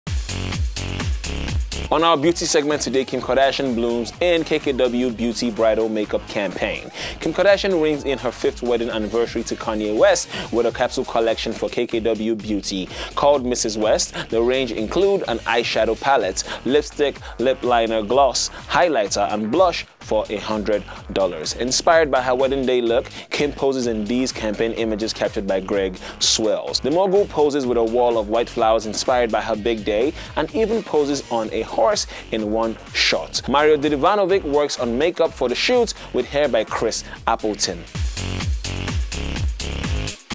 Kim Kardashian Blooms In KKW Beauty Bridal Makeup Campaign
Kim Kardashian rings in her fifth wedding anniversary to Kanye West with a capsule collection for KKW Beauty called ‘Mrs. West‘.

 The range includes an eyeshadow palette, lipstick, lip liner, gloss, highlighter and blush for $100.